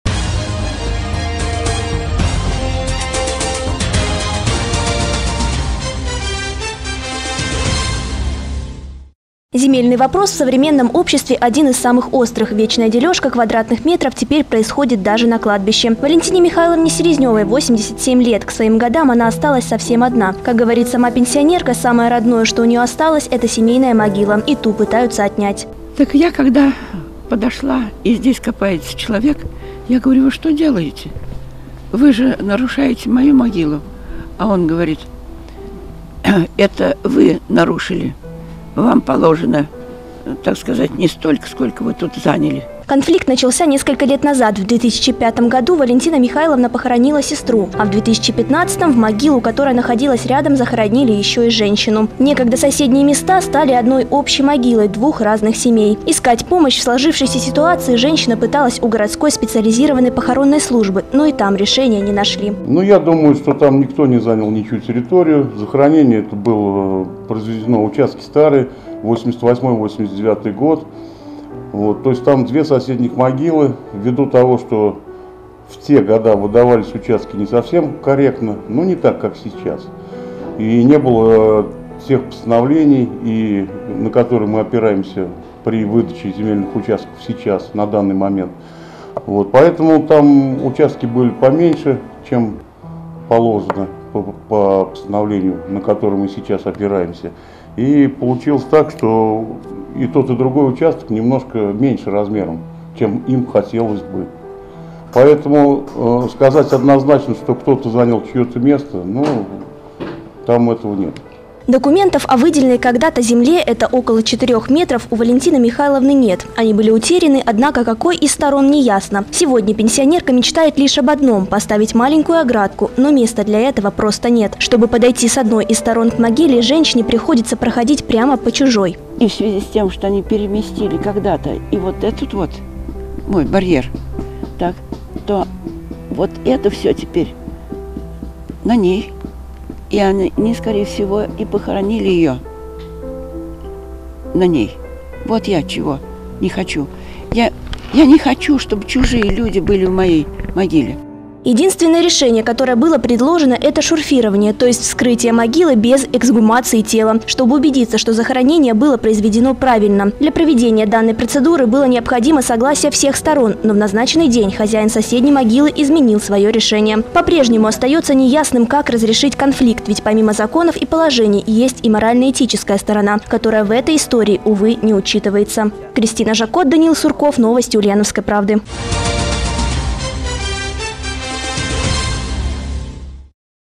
Чужие люди в могиле. На Ишеевском кладбище не могут поделить участки
Земельный вопрос в современном обществе один из самых острых. Вечная «делёжка» квадратных метров теперь происходит даже на кладбище.